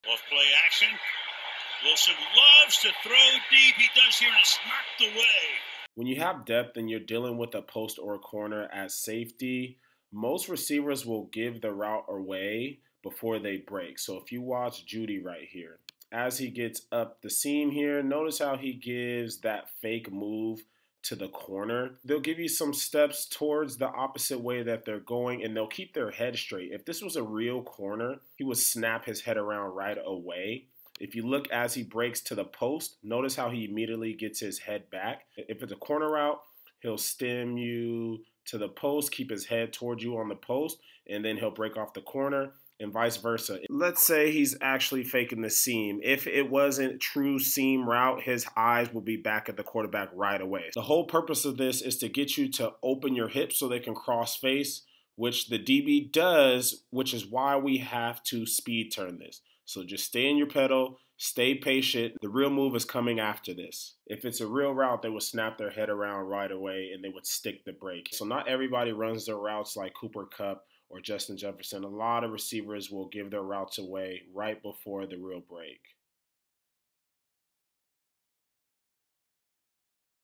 Watch This To Guard DEEP ROUTES…
In this video I breakdown HOW DBS CAN GUARD DEEP ROUTES. I hope you enjoy!